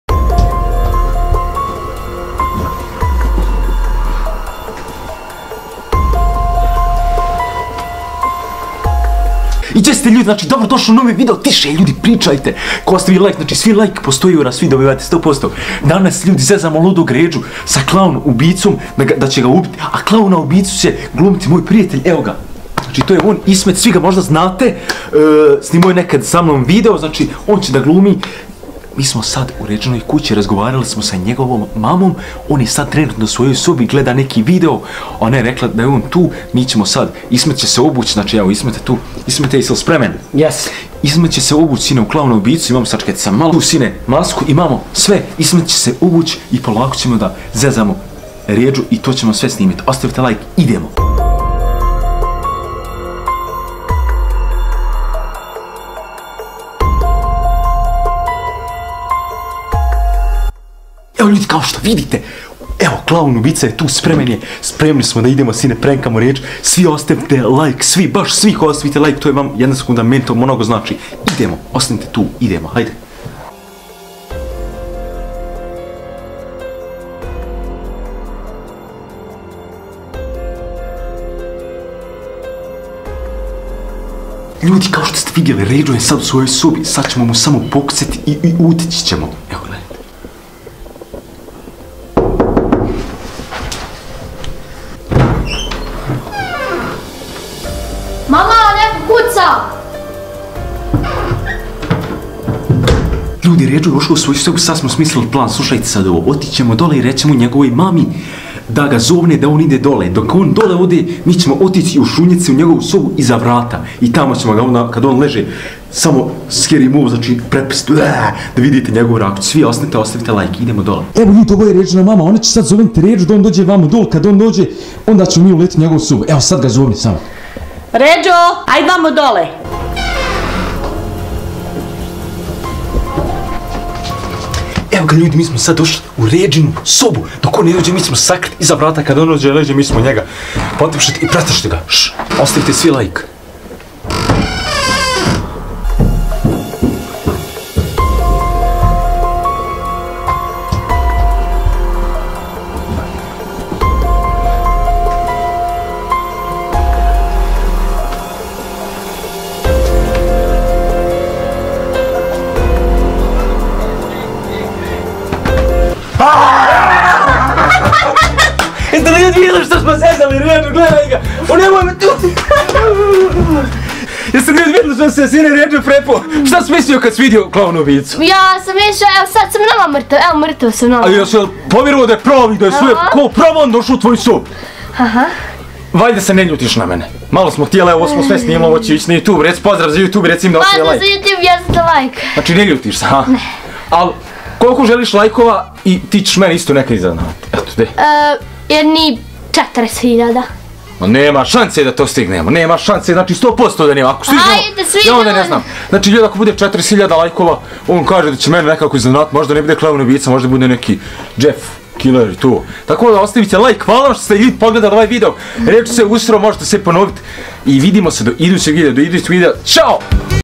KLOVN UBICA Napao REDŽU i Uradio OVO ! Prank!
Lajkujte video i pretplatite se na ALDIN KANAL !

U današnjem videu ALDIN i REDŽO rade jako cool i zabavne stvari !